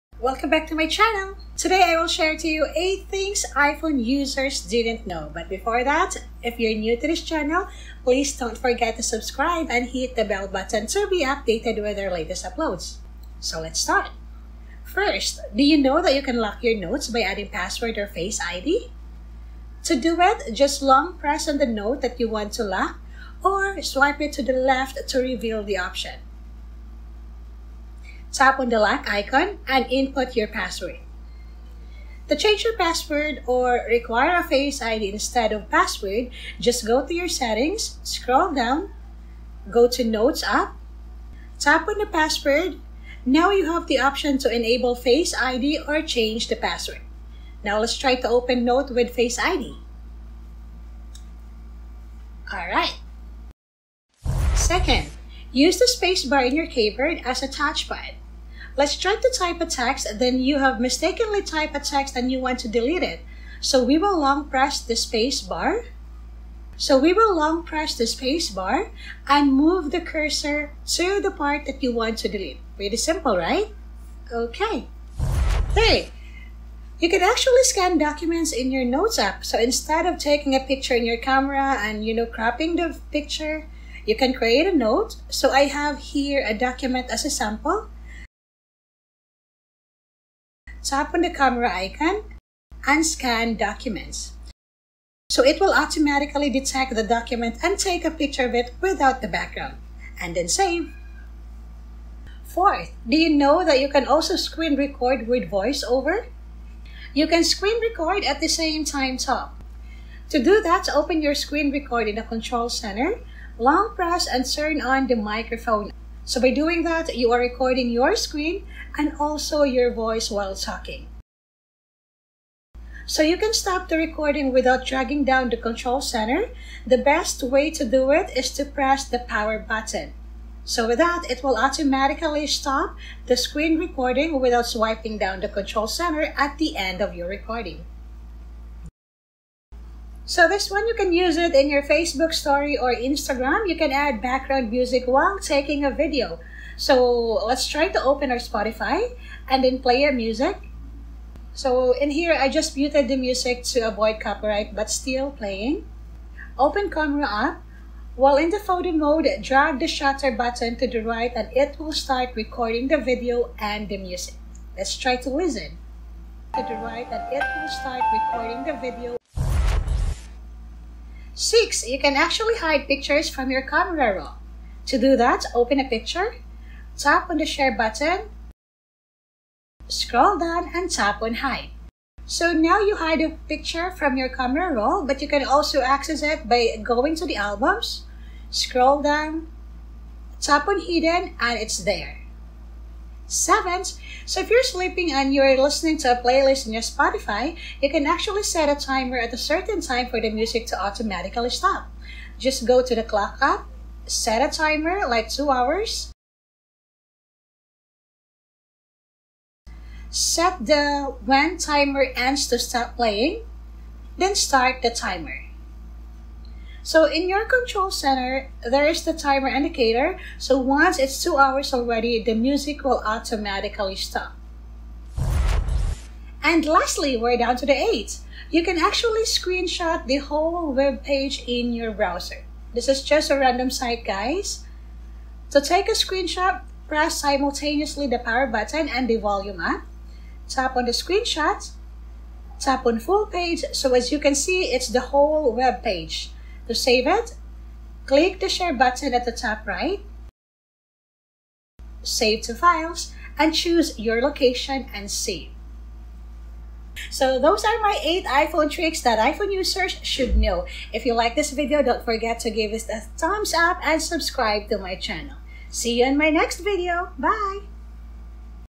Hidden Features of iPhone 11, Pro, Pro Max - Tips and Tricks 2020
8 cool things new iPhone users didn’t know.
Some are old features but are worth mentioning.

1. Lock Notes with Password or FaceID 00:14
2. Use Space Bar as a Touch Pad 00:55
3. Scan Documents using Notes App 01:16
4. Screen Record with Voice Over 01:42
5. Add Background Music While Taking a Video 02:20
6. Hide Pictures in Camera Roll 02:50
7. Set a Timer to Stop Music from Playing 03:15
8. Screen Shot a whole Web Page 03:46